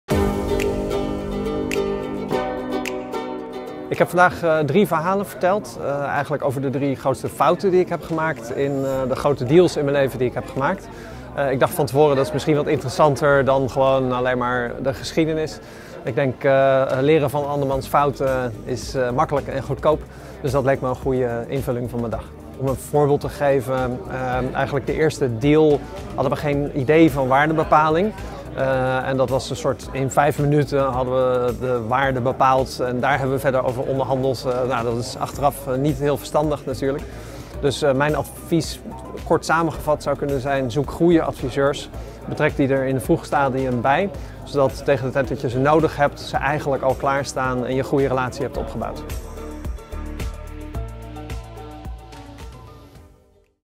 The Next Deal - Boris Veldhuijzen van Zanten (The Next Web)
Interview with Boris Veldhuijzen van Zanten (founder The Next Web) at The Next Deal - event Van Doorne / The Next Web.